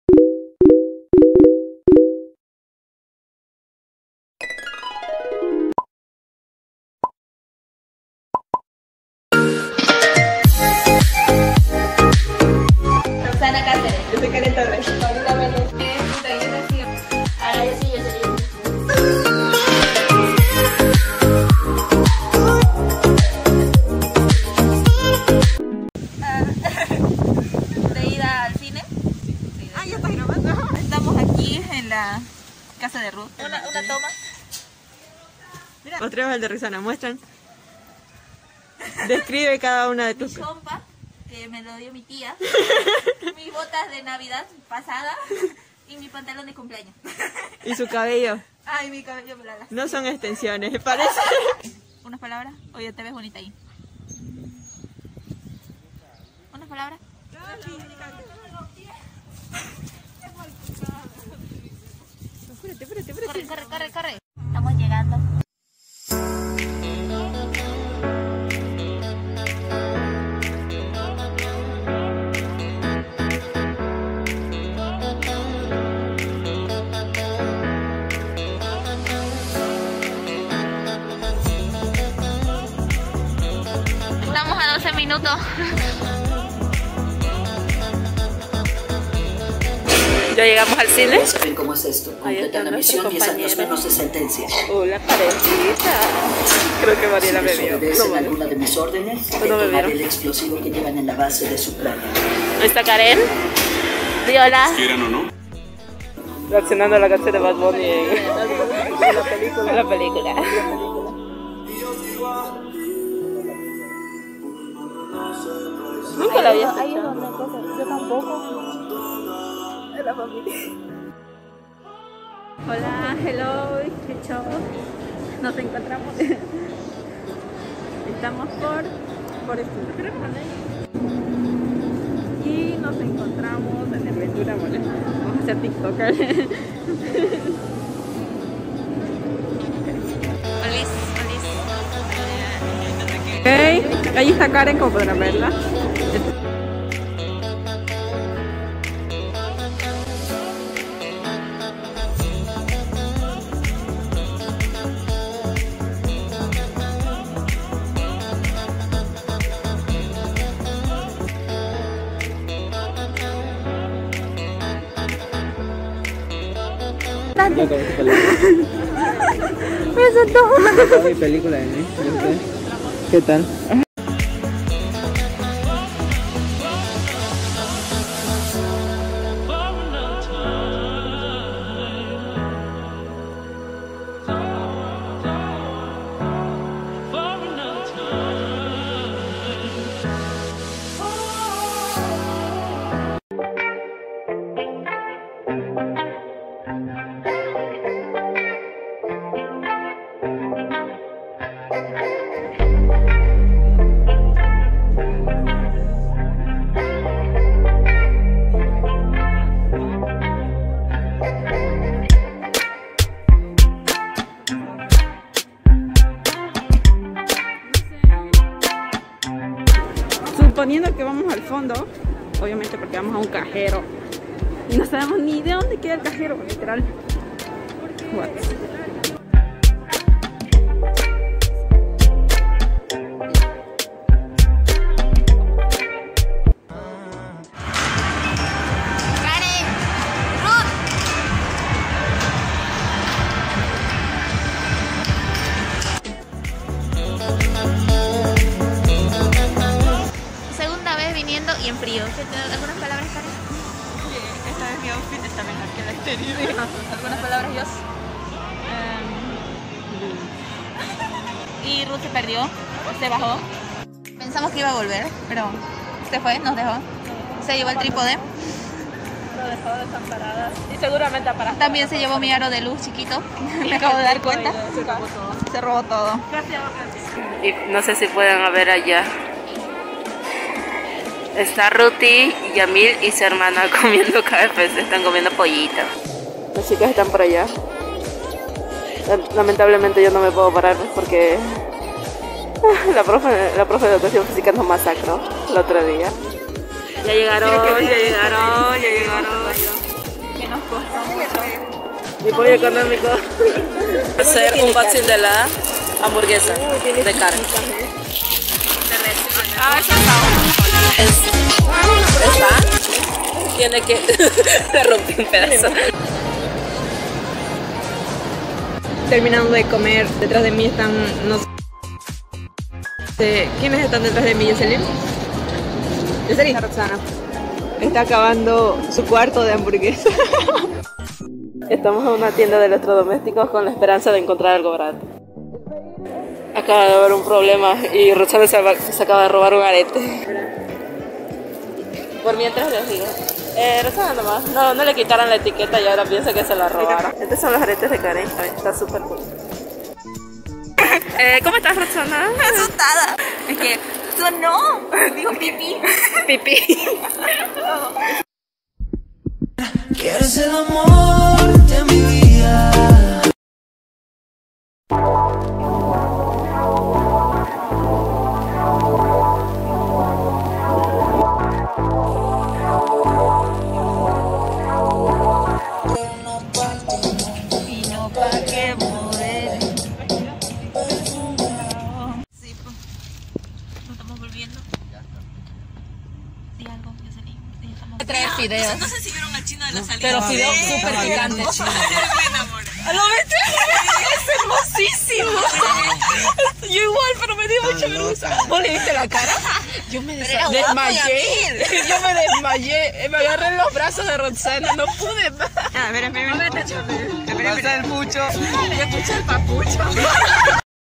Una tarde en el centro comercial* VLOG (Ruth se pierde)
Tuvimos una muy divertida tarde entre amigos...